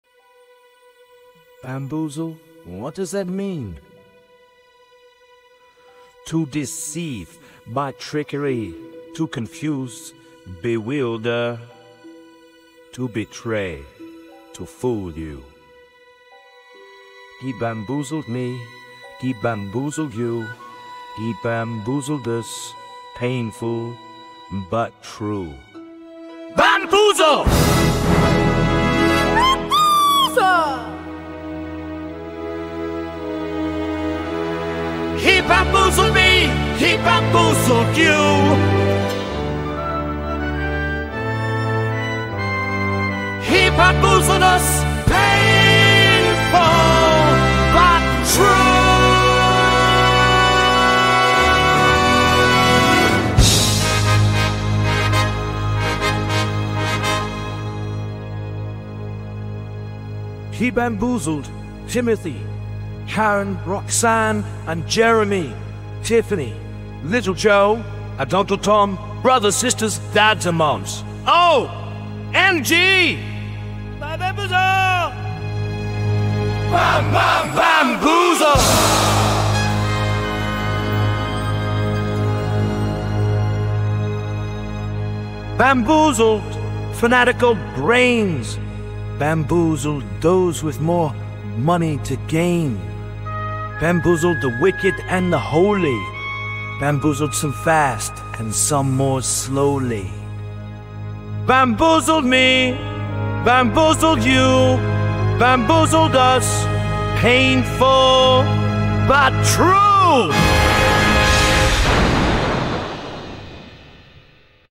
BAMBOOZLE The Art of Deception
Trump bamboozled 30 percent of the USA, but it's dropping fast.
To deceive or dupe; hoodwink. synonym: deceive.
To confuse; bewilder.
To deceive by trickery; to cajole by confusing the senses; to hoax; to mystify; to humbug.